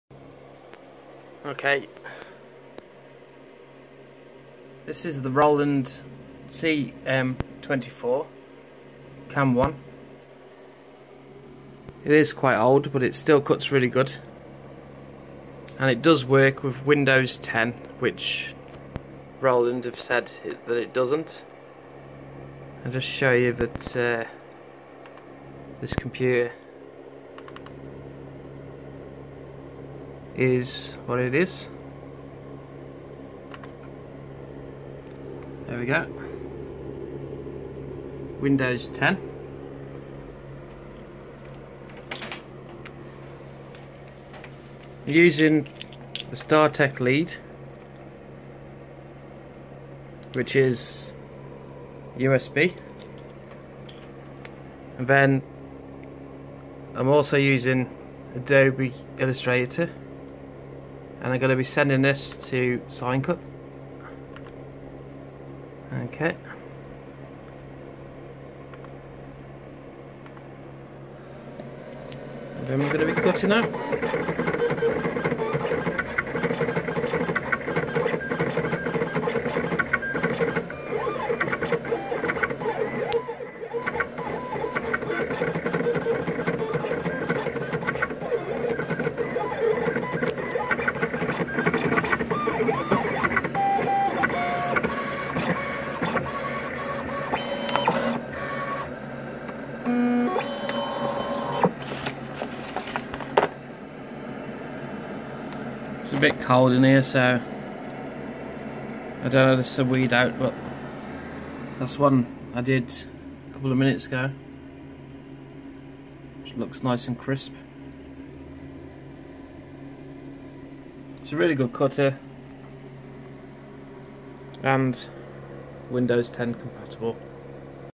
Roland CM-24 windows 10
Apparently the Roland CM-24 wont work with windows 10 since it isnt compatible according to Roland. Well with a startech lead, adobe illustrator and sign cut it is made possible. I am putting this on for the purpose of selling it on ebay.